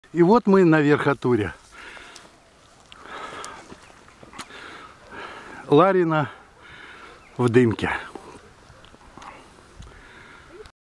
7 поход ''Ларино Придорожное'' 20161202 1036 MVI 0914
Сюжет похода из Ларино в Придорожное. (2 декабря 2016 года.)